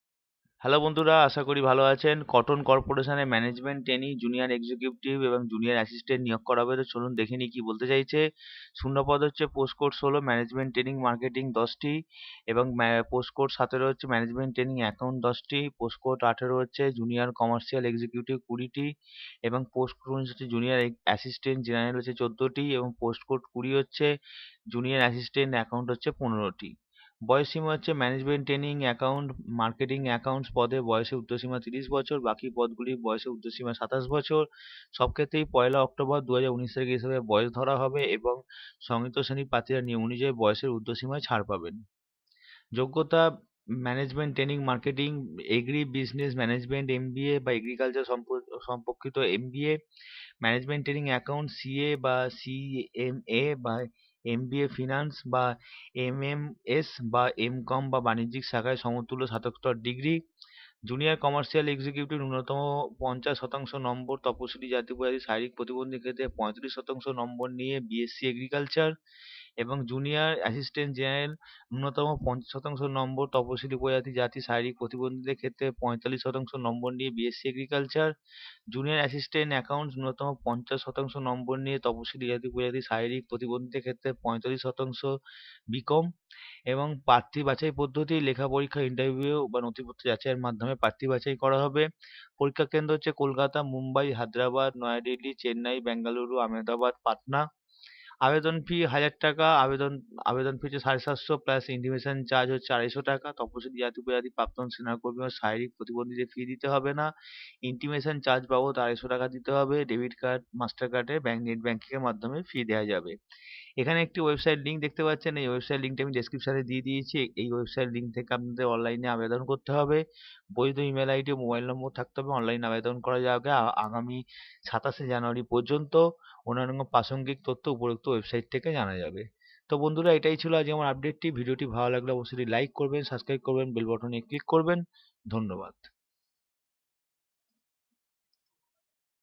Recruitment of Junior Assistant in Cotton Corporation
Five management trainees, junior commercial executives and junior assistants will be appointed at The Cotton Corporation of India Limited, a sub-division under the ministry's textile ministry. Applicants can apply online till January 27.

Management Trainee (Accounts): CA / CMA / MBA (Finance) / MMS / Mcom or equivalent Masters Degree in Commerce.

Junior Commercial Executive: BSc Agriculture with a minimum of 50 percent marks (45 percent number for Scheduled Castes / Tribes and Physically Handicapped).

Junior Assistant (General): BSc Agriculture with a minimum of 50 percent marks (45 percent for the Scheduled Castes / Tribes and Physical Disabilities).

Junior Assistant (Accounts): With a minimum of 50 percent marks (45 percent in the case of Scheduled Castes / Tribes and Physical Disabilities).